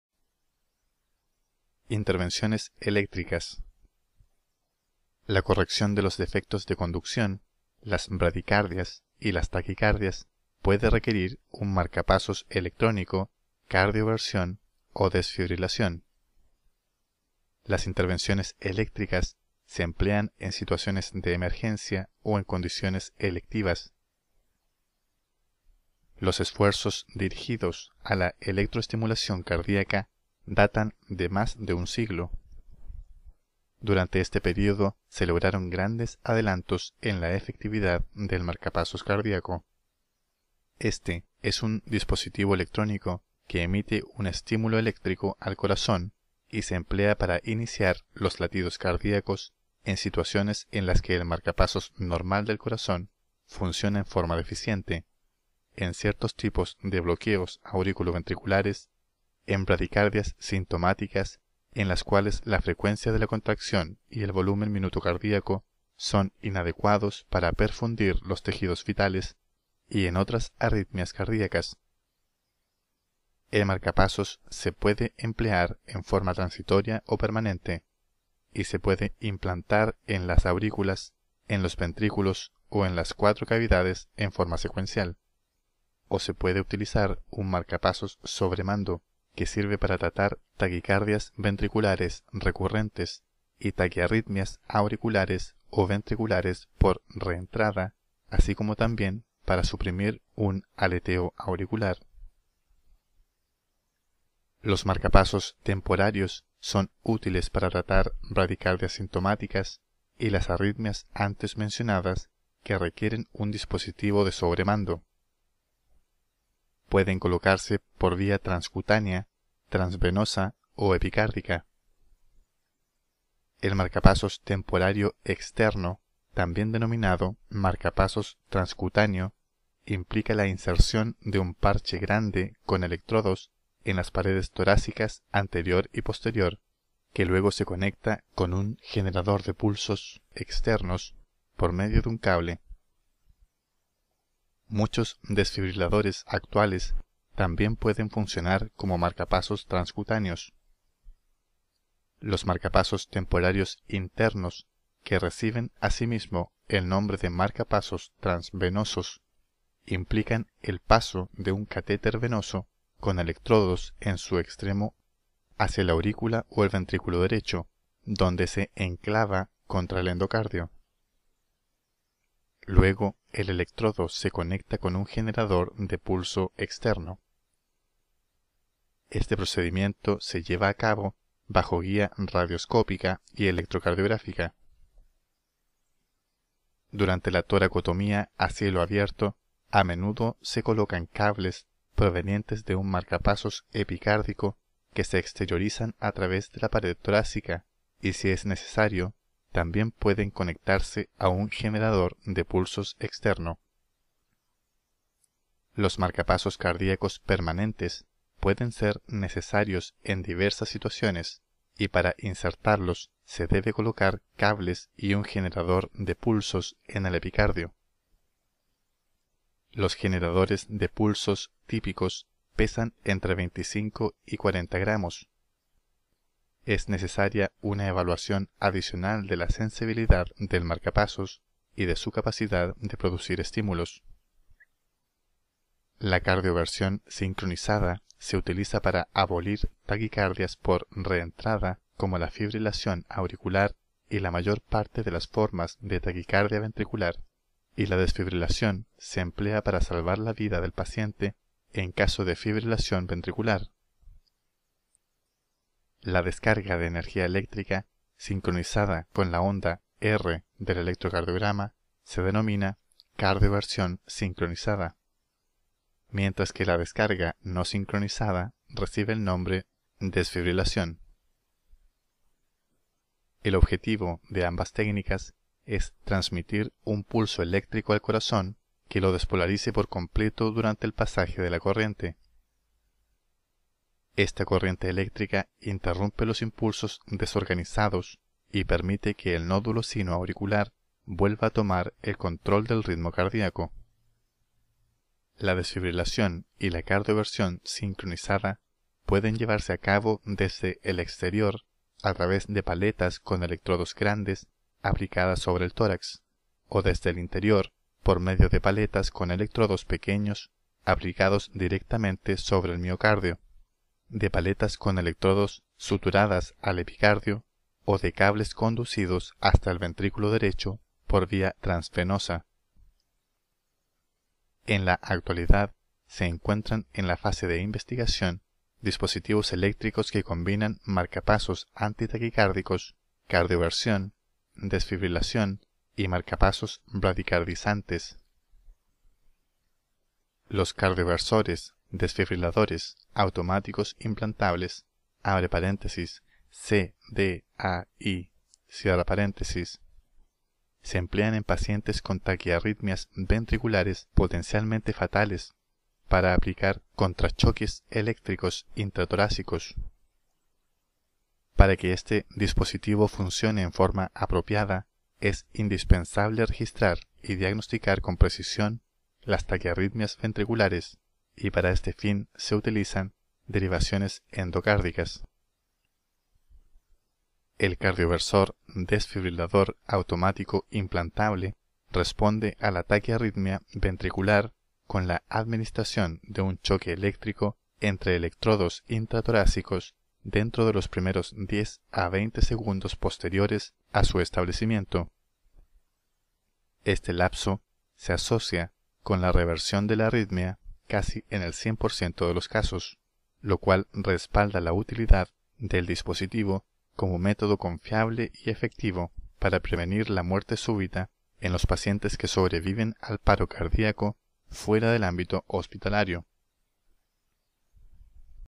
Intervenciones eléctricas - Trastornos del ritmo y de la conducción cardíacos
Lecturas sección FisiopatologíaPorthC 27 "Trastornos del ritmo y de la conducción cardíacos".